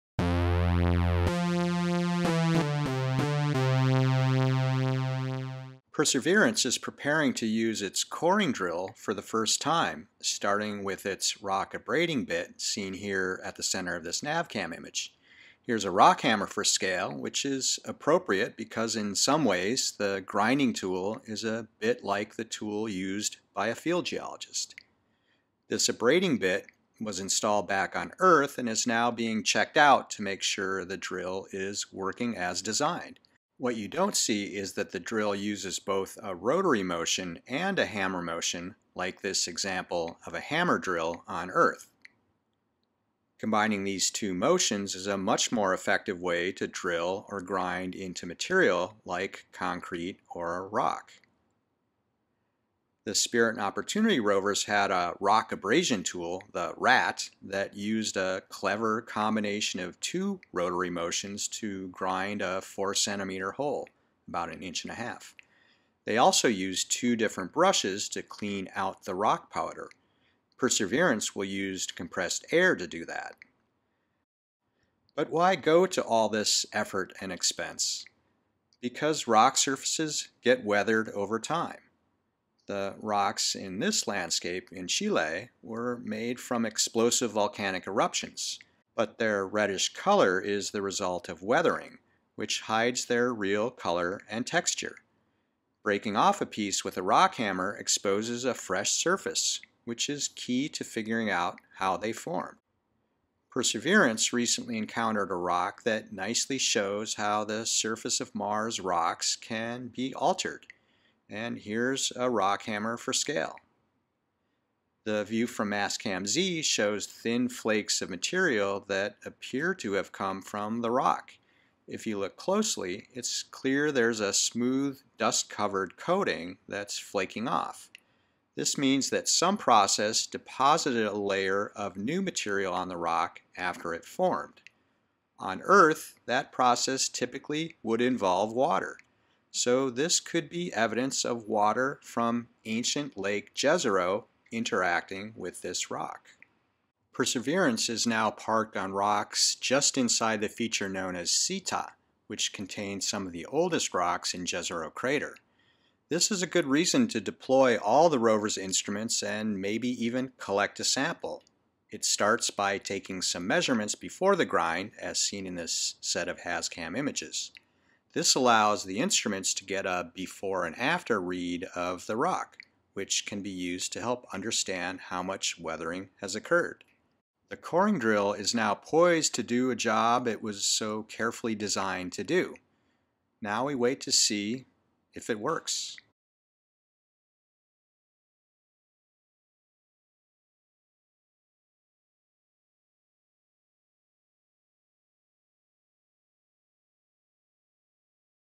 Perseverance rover is getting ready to grind
Episode 15
Mars rovers can't be equipped with a rock hammer, the most basic tool of field geologists.  But Perseverance may have something even better, a hammer drill with a custom abrading bit.  It's about to try it out for the first time on ancient rocks in Jezero crater.